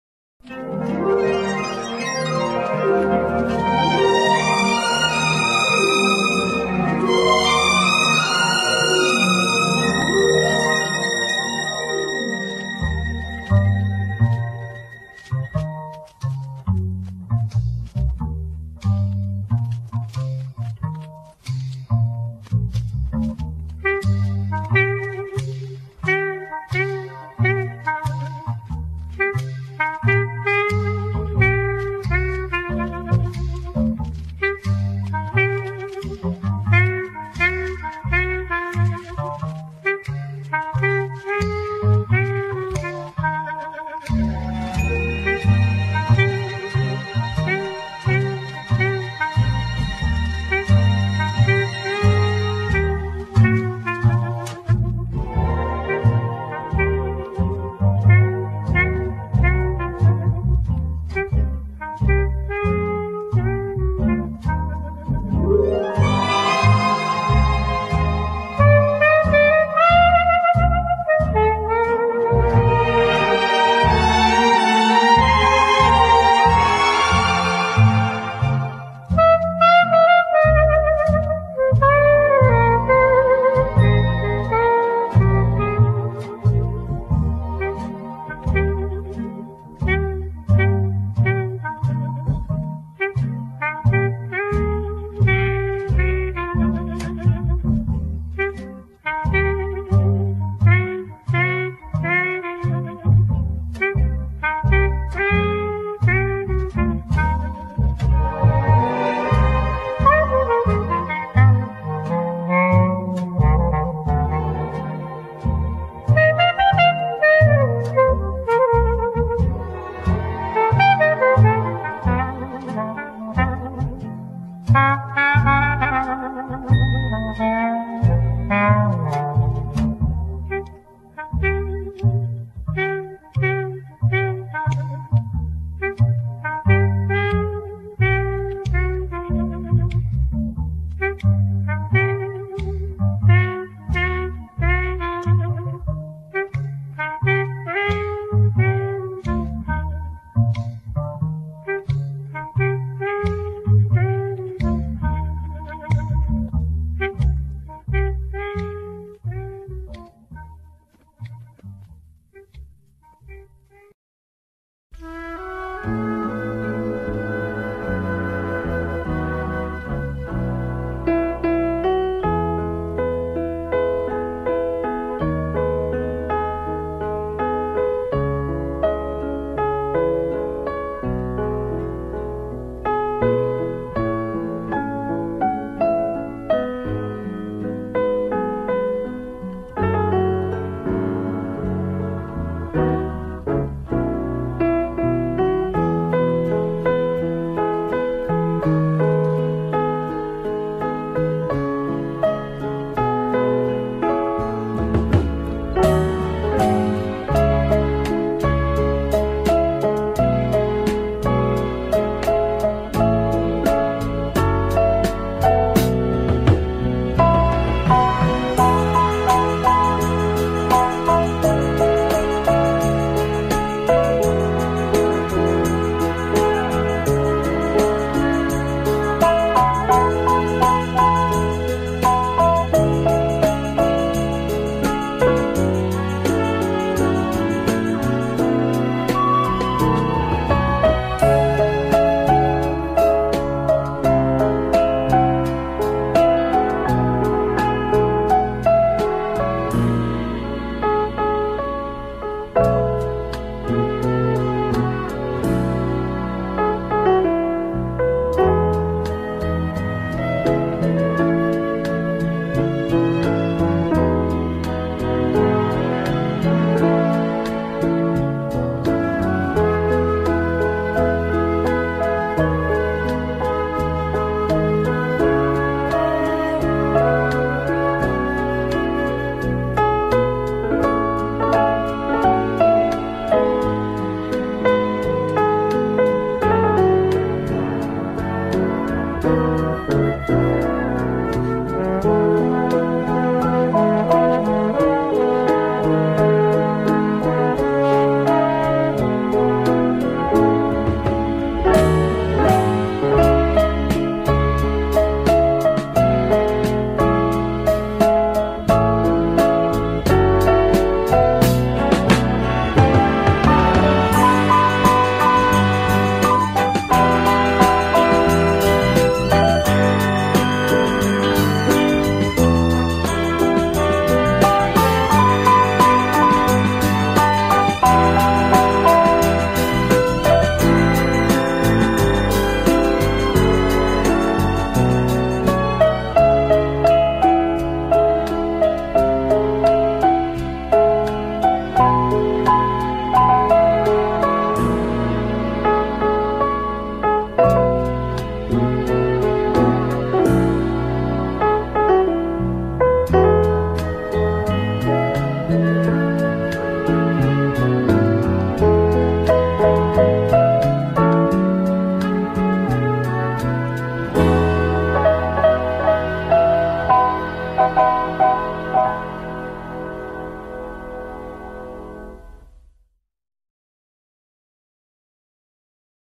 NY Botanical Gardens Tram Ride 051309
NY Botanical Gardens Tram Ride 051309 with pictures taken from a moving Tram